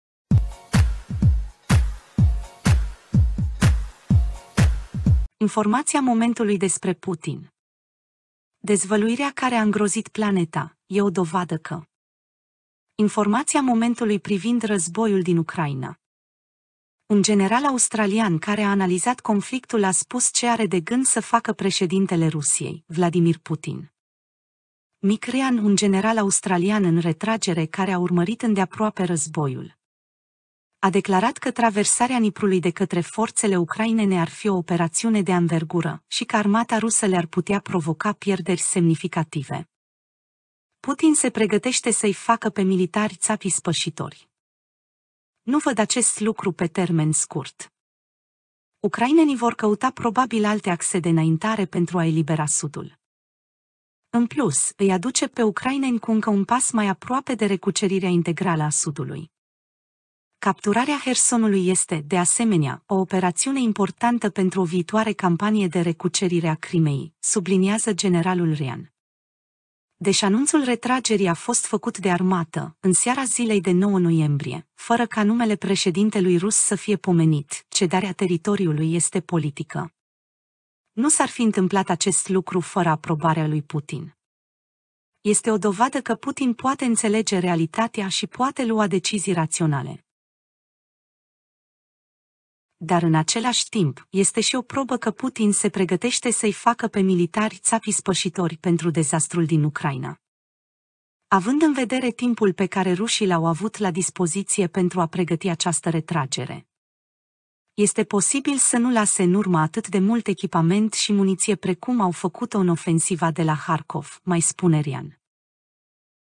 Informaţia momentului despre Putin! Dezvăluirea care a îngrozit planeta
Mick Ryan, un general australian în retragere care a urmărit îndeaproape războiul, a declarat că traversarea Niprului de către forțele ucrainene ar fi o operațiune de anvergură și că armata rusă le-ar putea provoca pierderi semnificative.